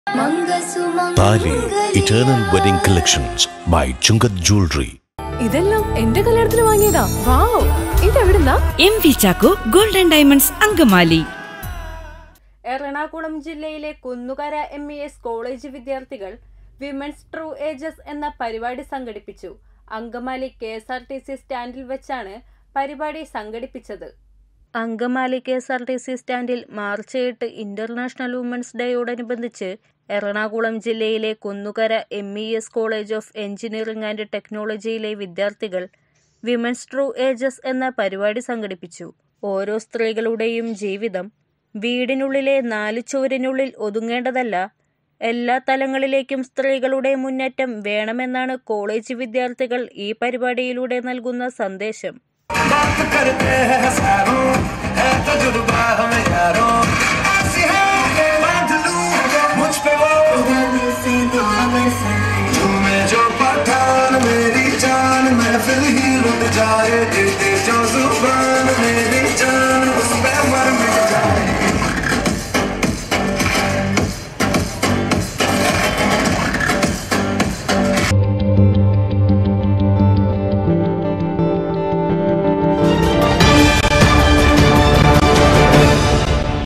എംഇഎസ് കോളേജ് വിദ്യാർത്ഥികൾ വിമൻസ് ട്രു ഏജസ് എന്ന പരിപാടി അങ്കമാലി KSRTCൽ വെച്ച് നടത്തി
MAR 1 ,2023#actvnewsangamaly